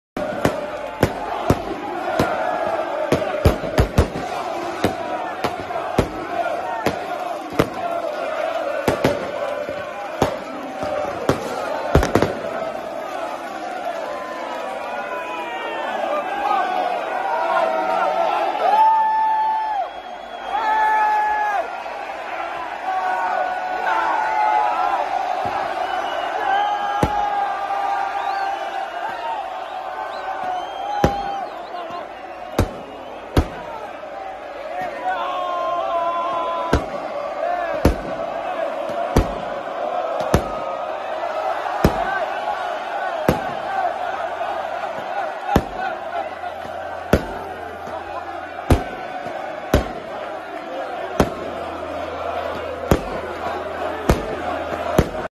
SC Cambuur promoted to Eredivisie. 16/04
Cambuur vs Helmond Sport.
Madness in Leeuwarden! SC Cambuur supporters & players celebrating their promotion to the Eredivisie tonight!